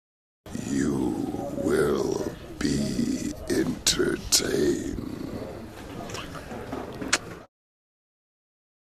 Vin Diesel and Deepika Padukone's FIRST video together: Here's what he said! | itimes
After actor Vin Diesel, who will essay the role of Xander Cage in the film, shared the first look with Deepika Padukone on Instagram, he has now given his fans a sneak peek into a 'promise'. The video which features Deepika is just another way to sustain the viewers’ interest in the much-awaited film.